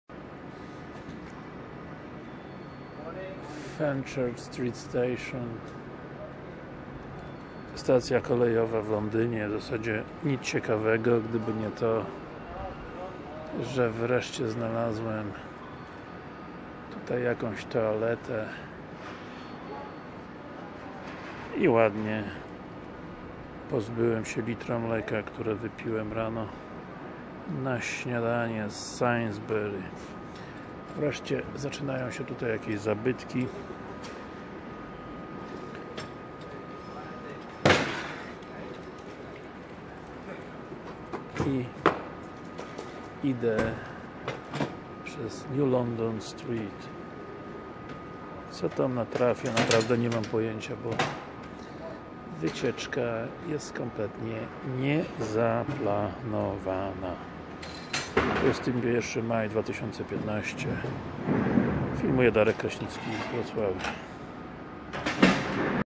Wycieczka po Londynie w 2015 roku
Spacer po Londynie po zupełnie przypadkowych ulicach.